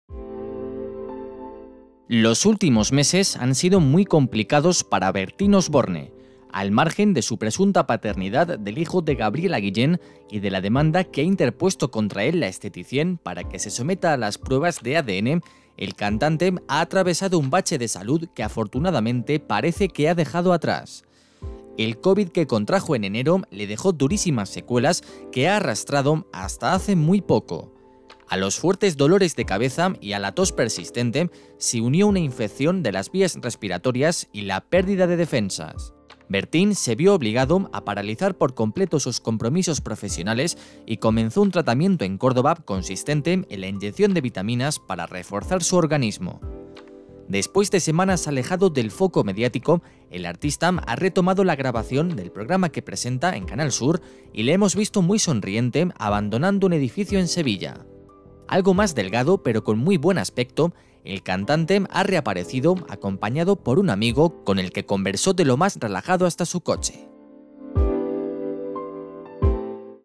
Bertín Osborne reaparece con muy buen aspecto tras superar sus problemas de salud
Los últimos meses han sido muy complicados para Bertín Osborne. Al margen de su presunta paternidad del hijo de Gabriela Guillén, y de la demanda que ha interpuesto contra él la esteticién para que se someta a las pruebas de ADN, el cantante ha atravesado un bache de salud que afortunadamente parece que ha dejado atrás.